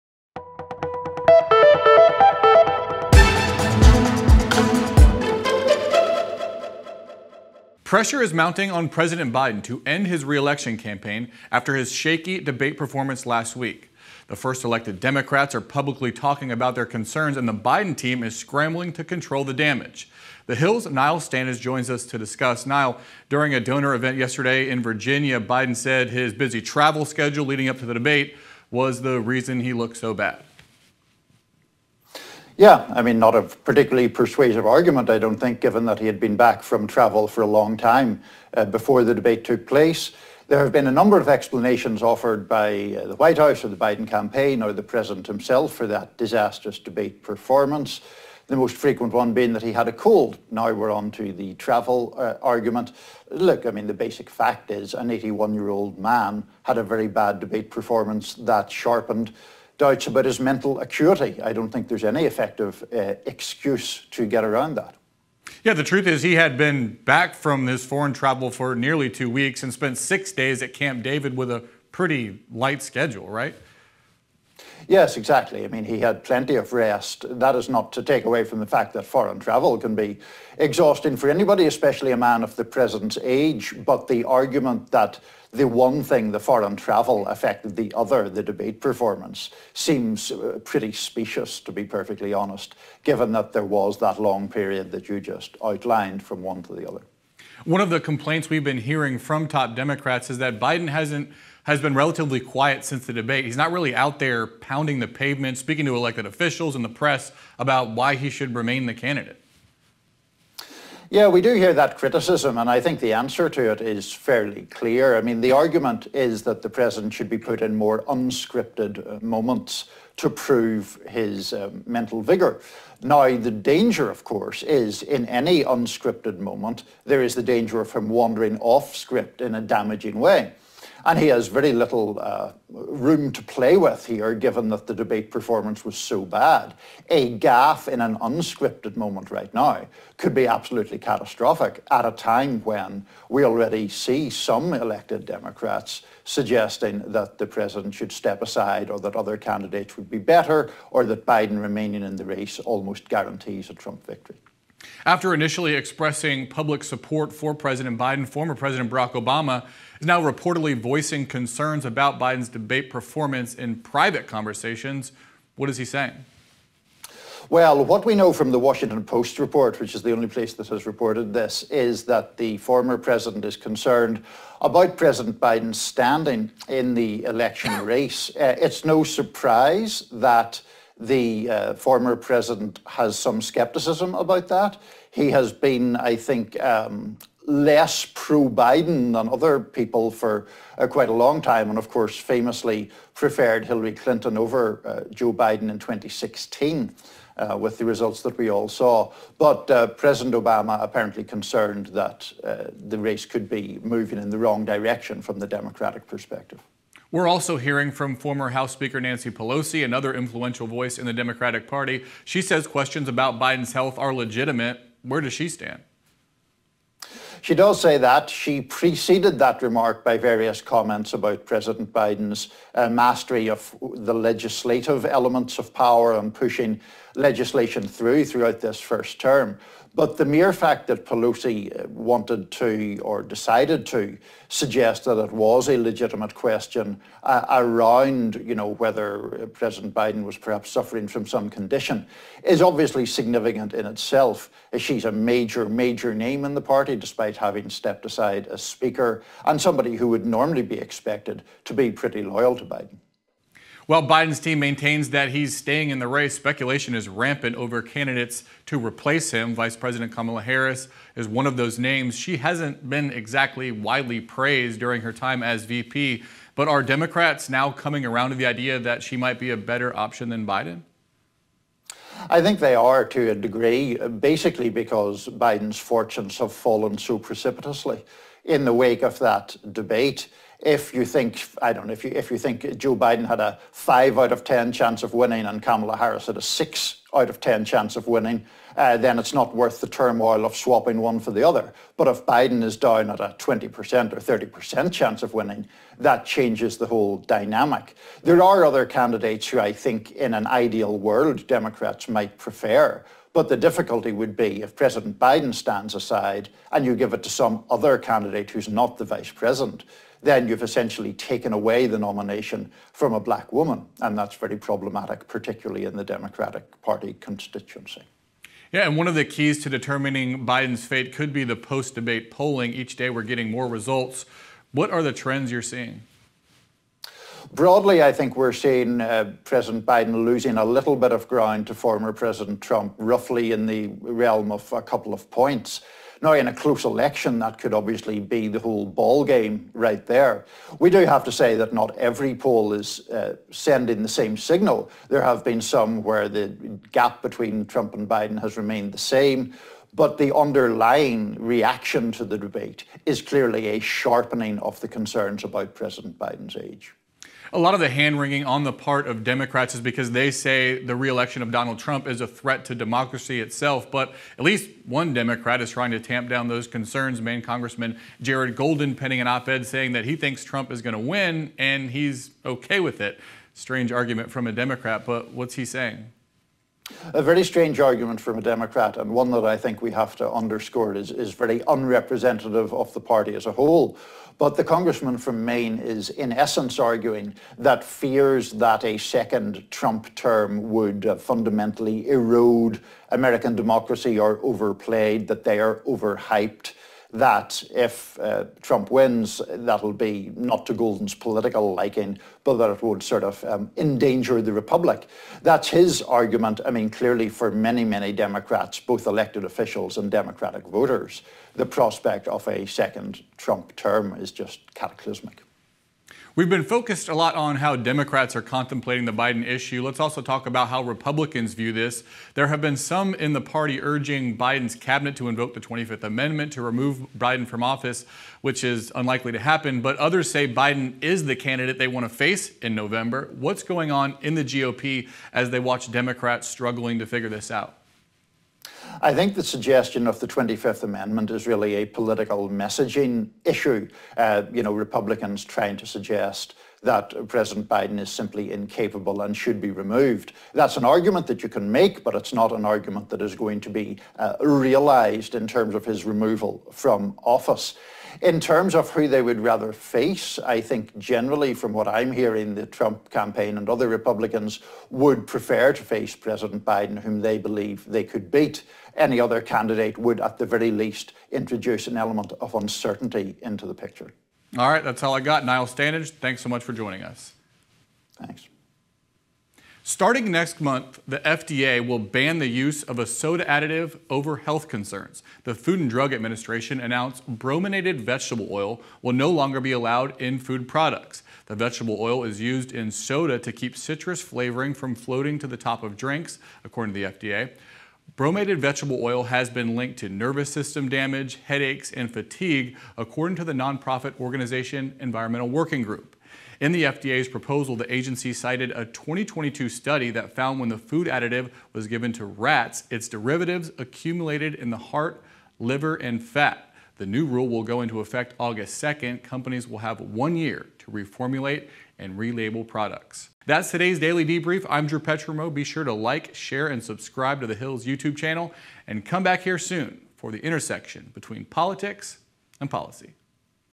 Biden: "I Almost Fell Asleep On Stage" The President Blames Foreign Travel For Sleepy Debate Debacle
The Hill’s Drew Petrimoulx chats with White House columnist Niall Stanage on Democrats’ concerns regarding Biden as the presidential nominee. A growing number of top Democrats privately and publicly are speaking out on the President's health and his 2024 chances. Several Republicans continue to float the idea Vice President Kamala Harris should invoke the 25th Amendment.

Plus, new polling could show troubling times ahead for President Biden. The FDA announces it will ban a soda additive that could be harmful to your health.

Follow The Hill on Instagram and X,